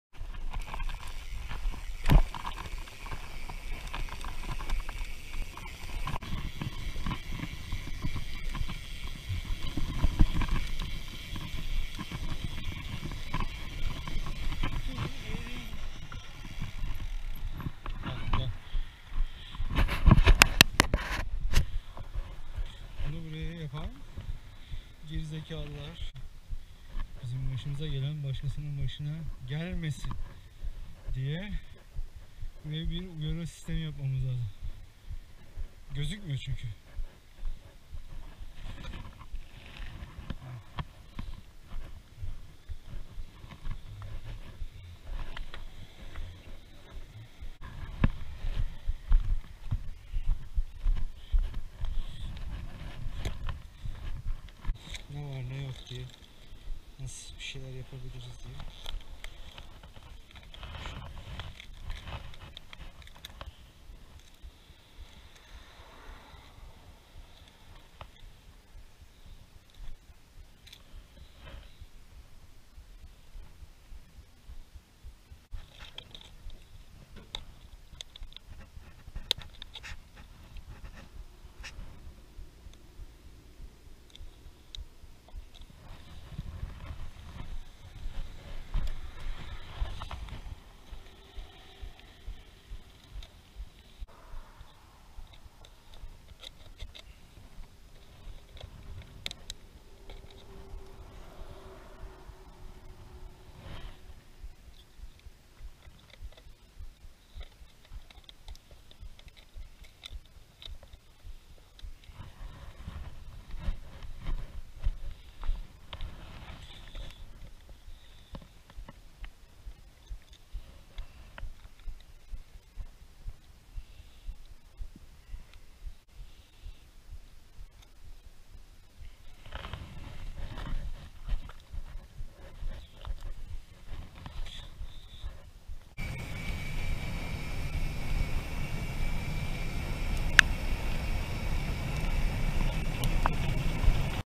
Patikalardaki Tehlikeli Dikenli Tellere Dikkat!!!
Doğa sporları ile uğraşan insanların her zaman geçtikleri veya ilk kez geçecekleri patikalar üzerinden dikenli tel geçirebiliyorlar bu arsanın sahibi yerini belirtmek istiyor fakat daha önceden açılmış halen kullanılmakta olan patikanın üzerinden bu şekilde dikenli tel geçirince ne gibi kazalara yol açacağını düşünemiyorlar aman siz dikkat edin kendinizi ve sonrakileri koruyun iyi sürüşler :)